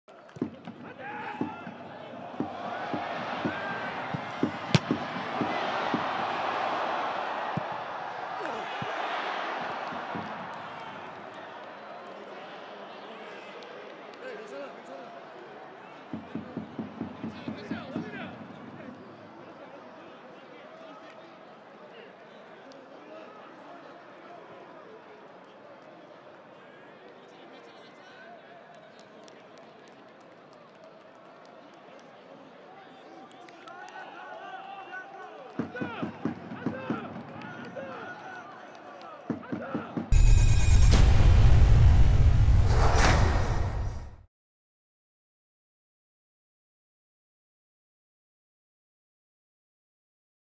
Long range thunderbolt from Adelaide's Jae-Sung Kim
During his side's 3-1 AFC Champions League win over Jeju United.